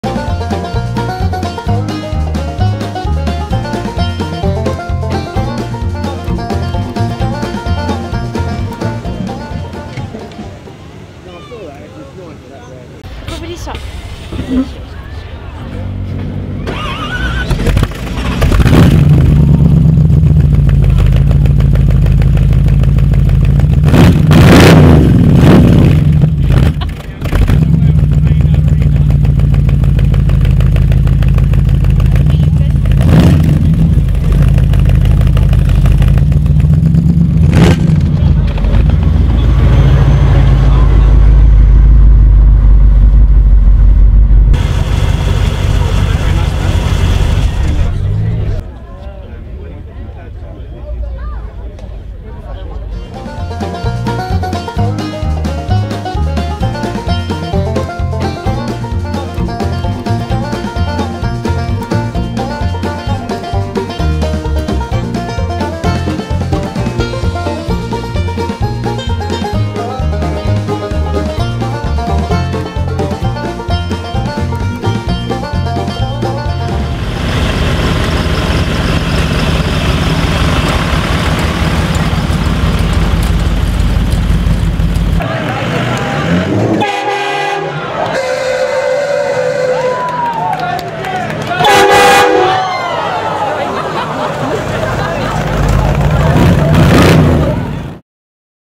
1949 Mercury Lead Sled (2nd Video)
Another look at 'The PHANTOM', Trevor Cowell's award winning custom 1949 Mercury Club Coupe. 519 YUG.

T J Motors.